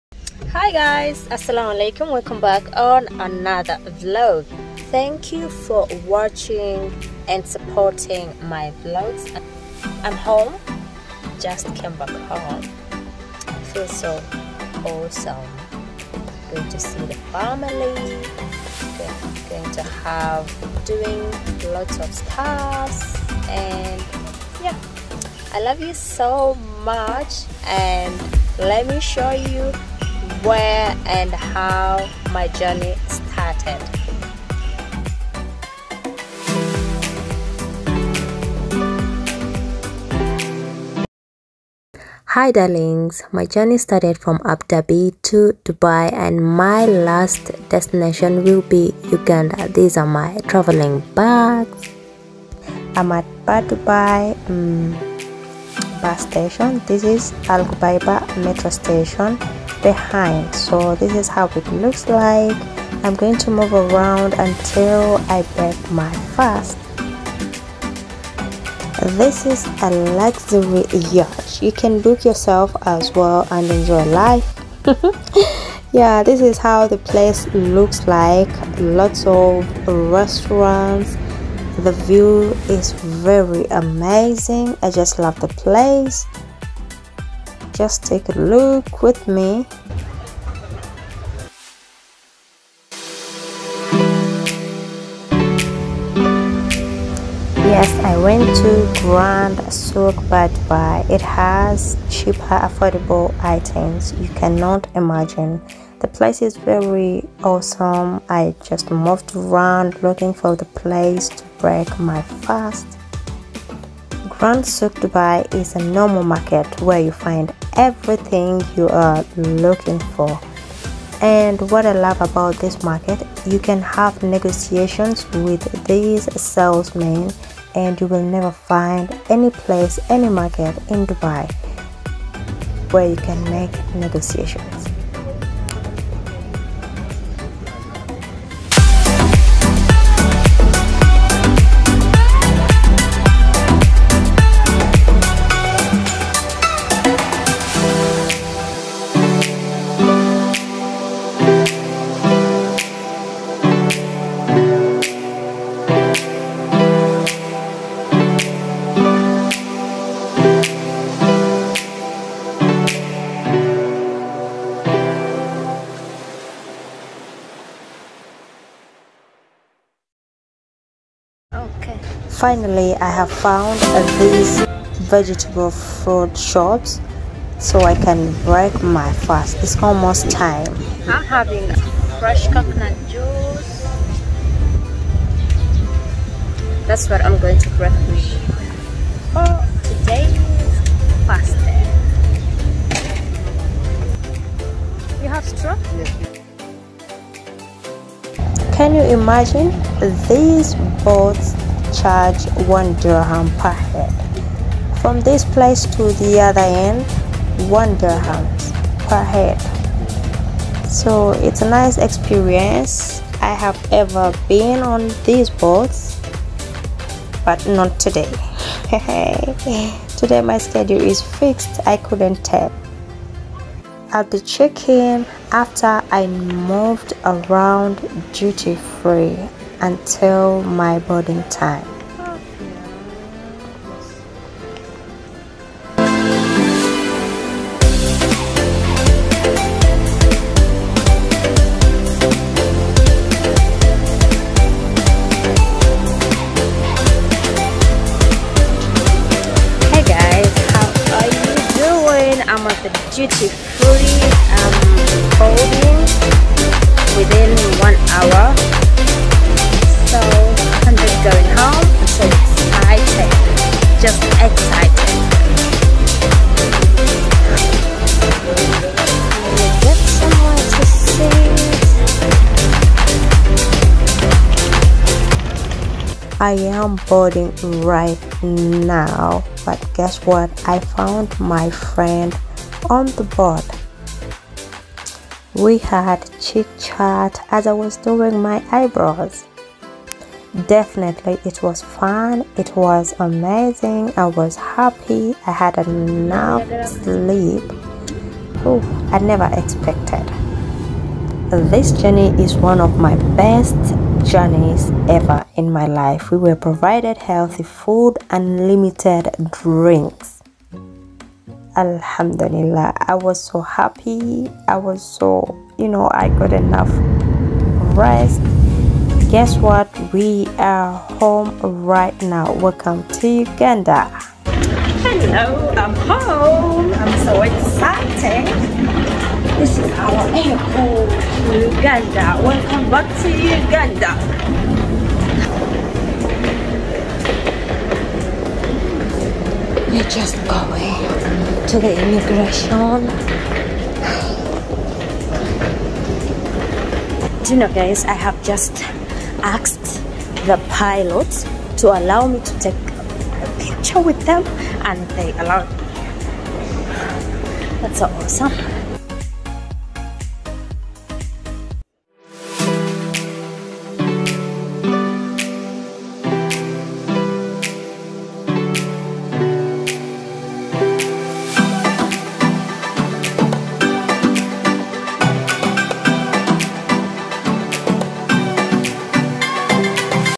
Travel Vlog: DUBAI to UGANDA🇺🇬
Hey Lovelies ,yes we all travel but travelling to home country is one of the Best feel one could not express. this time I travelled from Dubai to uganda ,where I enjoyed the whole journey, additional to that I met a friend on the plain where we chic-chatted ,slept off and had the best meal. thanks to flydubai